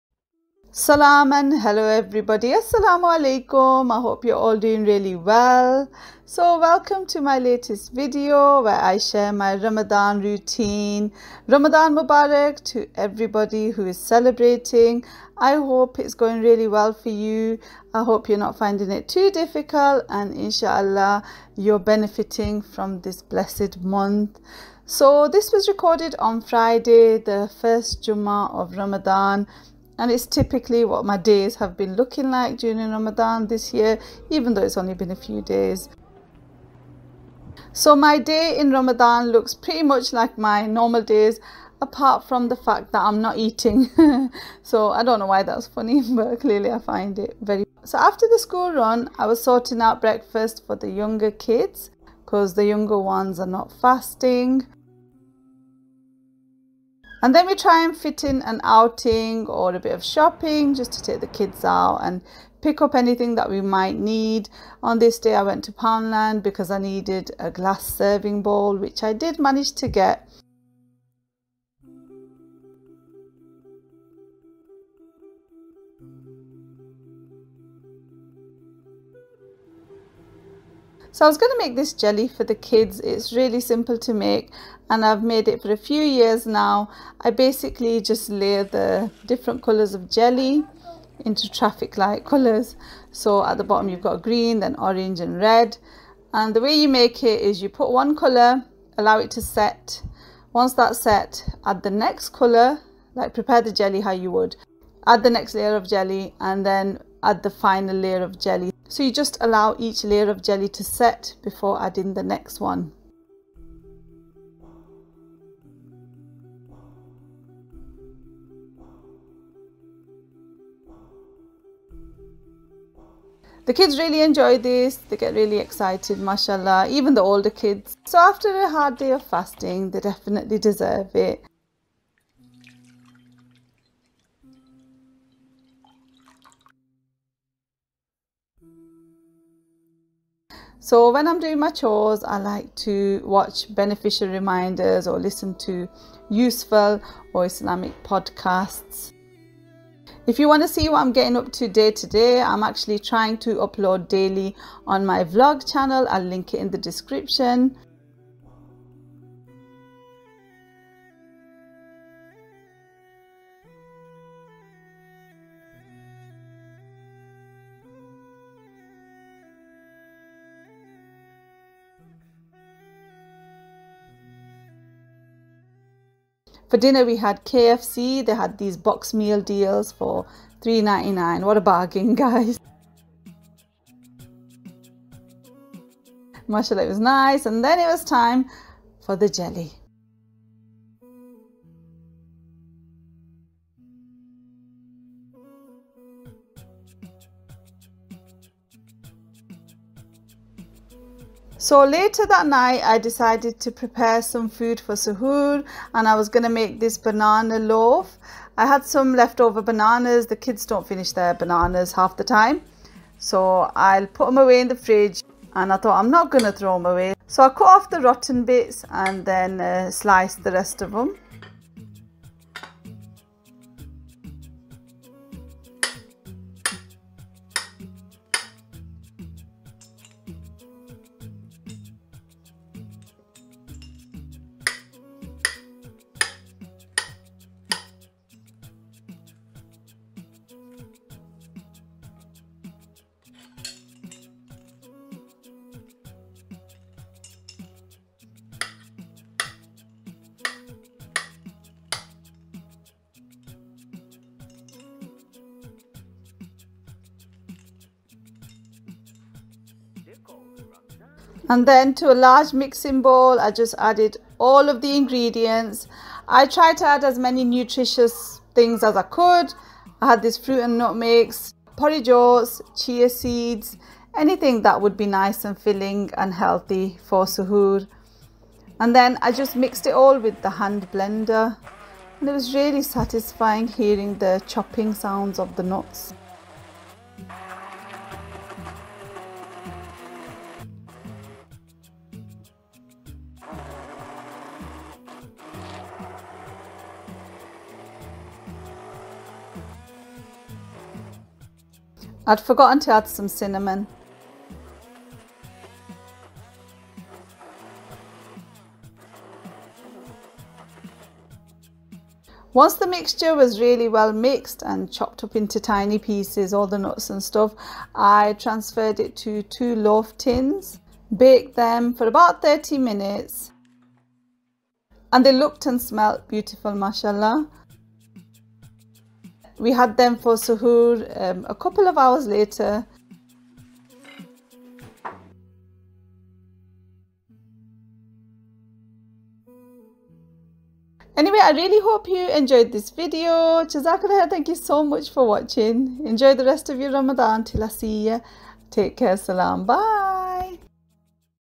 Ramadan routine & delicious nutritious banana loaf
In this video I share a day in my life (my routine) during Ramadan, and bake a delicious and nutritious banana loaf.

Banana loaf ingredients:
3 Bananas (sliced)
200g Light Brown Soft Sugar
135g Butter (melted)
2 Eggs
150g Porridge Oats
150g Plain Flour
100g Fruit & Nut Mix
1 tablespoon Baking Powder'
1 tablespoon Chia Seeds
1 teaspoon Vanilla Extract
1 teaspoon Ground Cinnamon
Pinch of Salt
Bake @ 180c/160c fan for 30 mins

Camera I use: Canon PowerShot G7X Mark ii Digital Camera

Try Amazon Audible